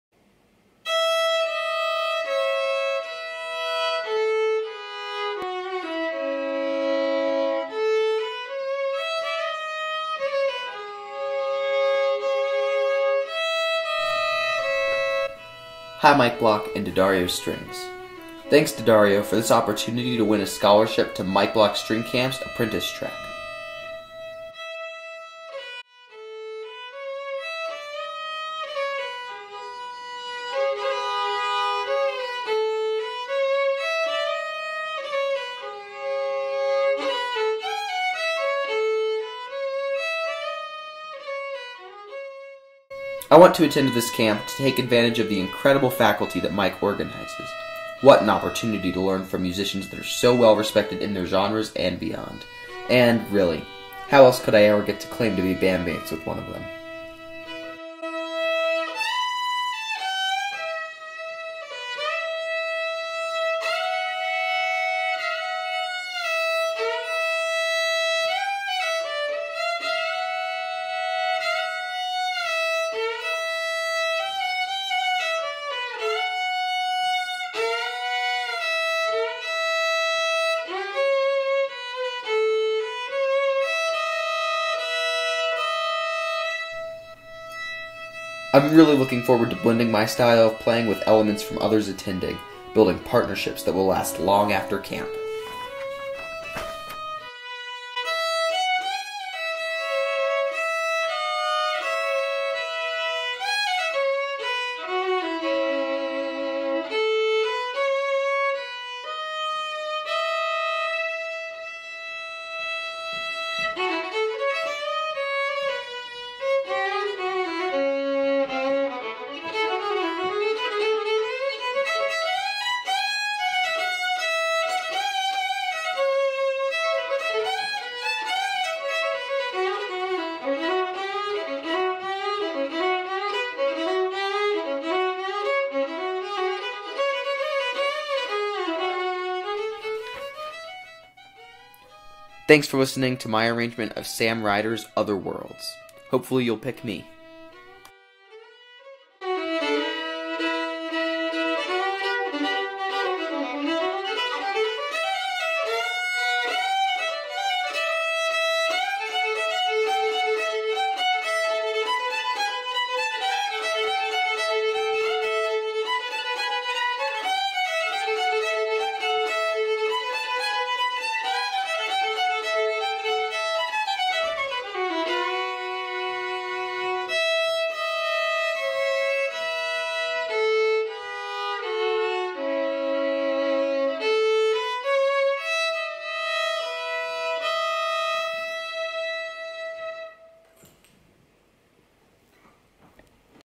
Mike Block String Camp 2018
My application to D'Addario Orchestral's scholarship to the 2018 Mike Block String Camp apprentice track.